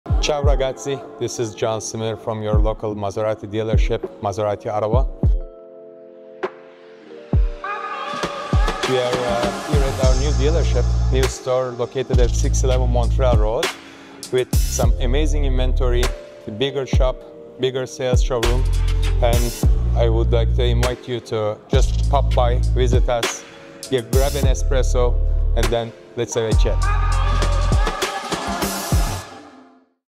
NEW LOCATION! Welcome to Maserati Ottawa | 611 Montreal Rd.
Assistant General Manager, John Cimir, welcomes you to the new Maserati Ottawa dealership. This short preview accentuates the beautiful new showroom and service centre that Maserati Ottawa now calls home. Come visit us at 611 Montreal Road, enjoy a fresh espresso, and witness everything the brand has to offer.